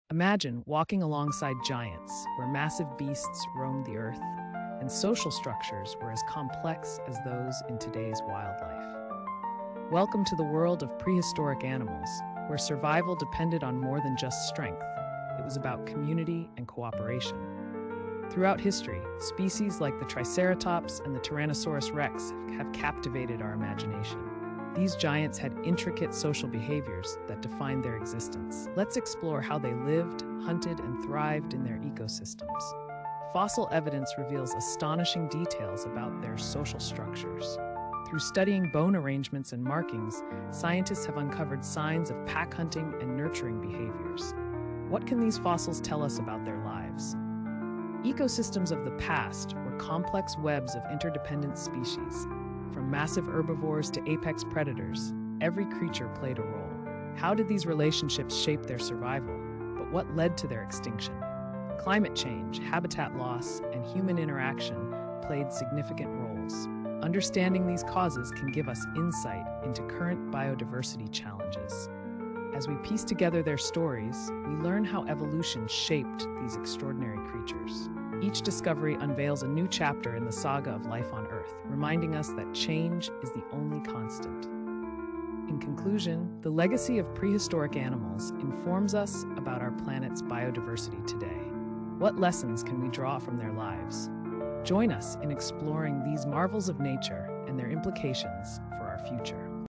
Giants of the Past: Unraveling the Complex Lives of Prehistoric Creatures and Their Ecosystems
Step back in time to discover the intricate social lives of prehistoric giants and the lessons they hold for us today. 🌍✨